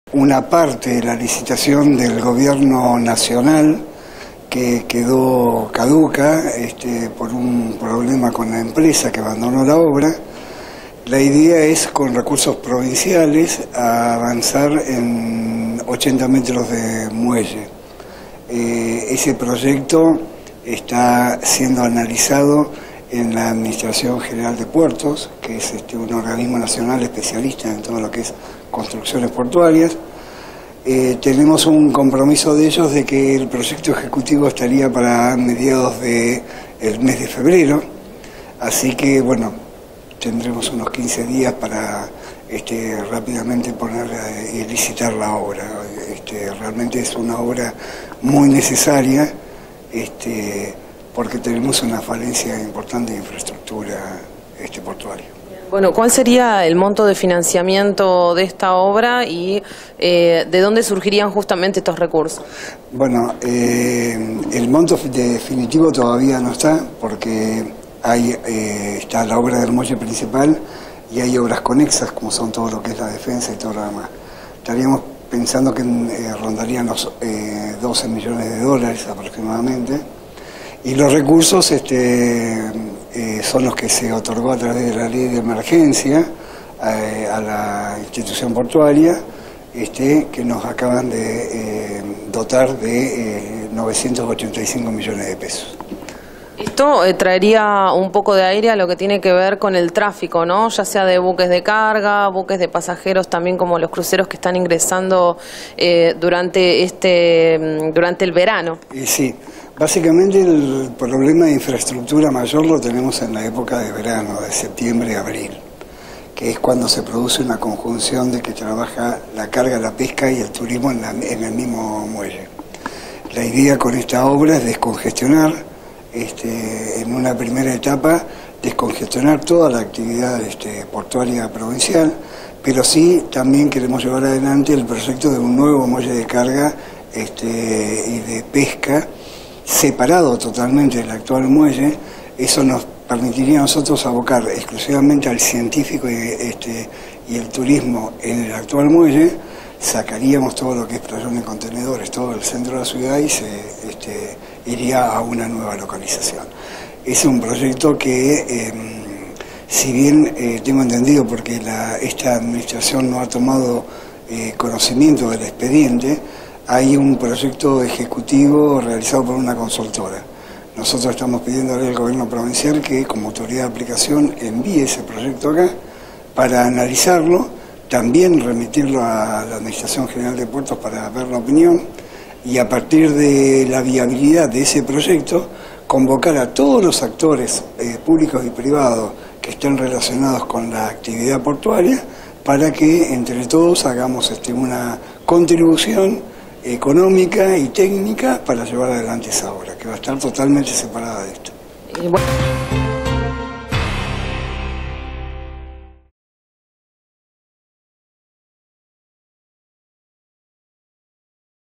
Llamado a licitación | Interés General | 09/01/2020 - TV2 Noticias
La dirección provincial de puertos pretende alargar 80 metros el muelle comercial